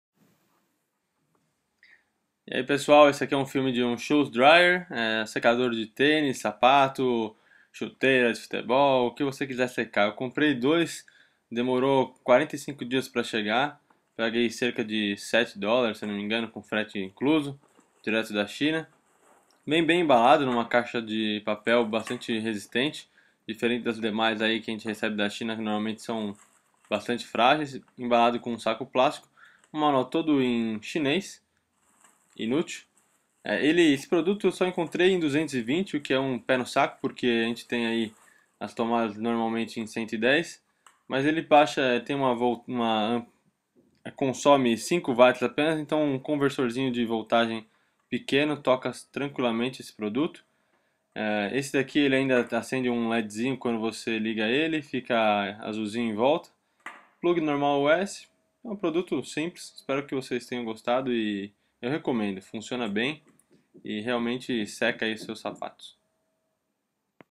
Shoes Drier
Shoes Drier comprado na China - por 5 ou 7 dolares. Produto funciona !